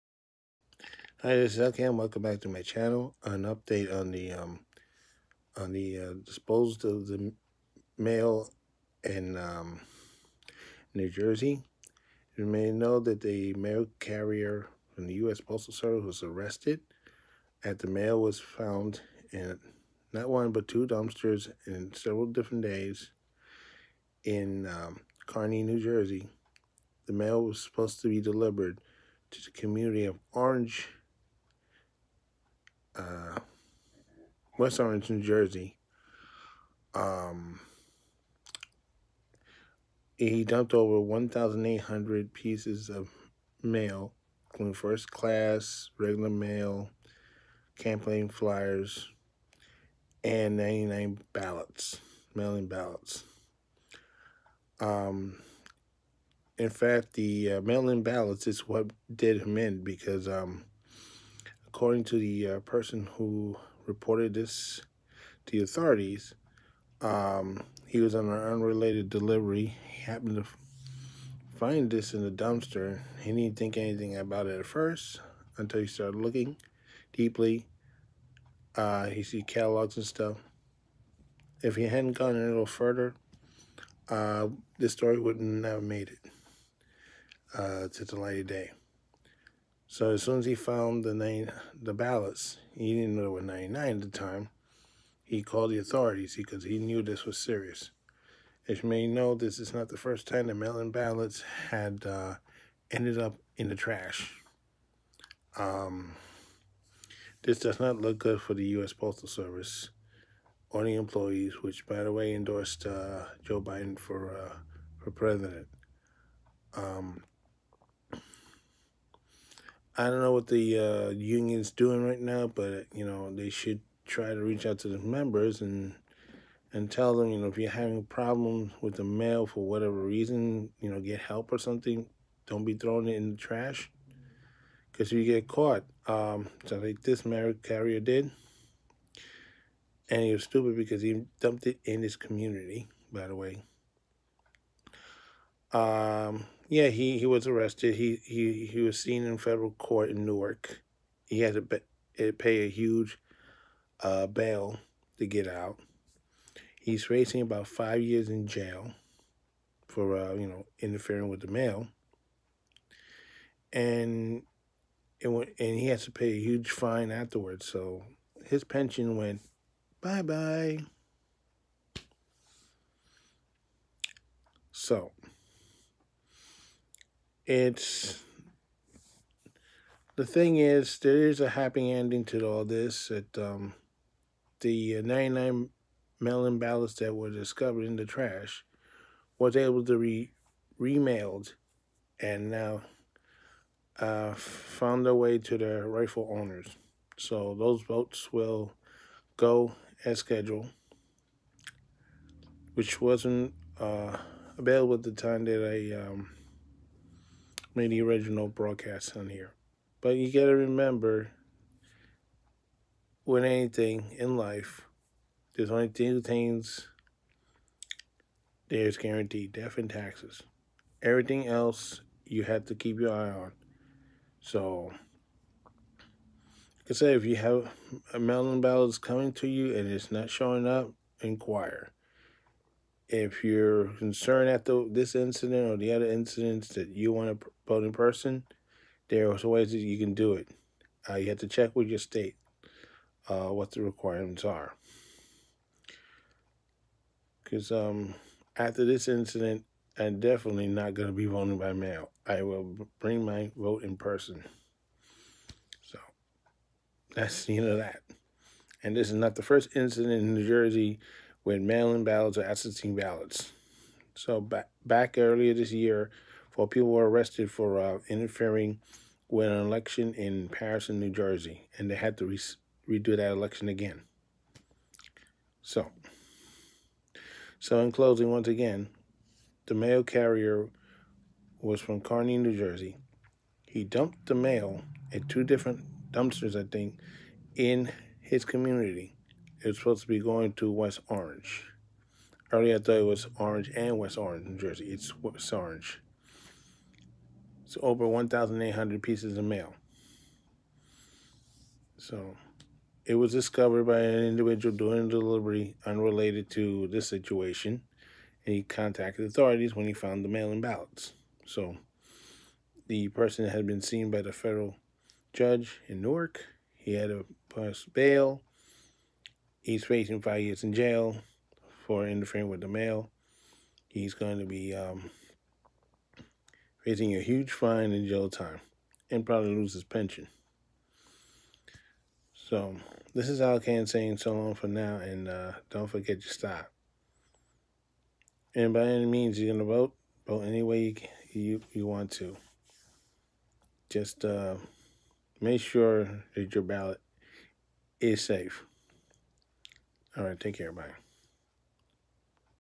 Update of US mail dump in trash usps employee arrested
An update of incident where a usps employee dumped over 1,850 pieces of mail that was supposed to be delivered to West Orange NJ including 99 mail in ballots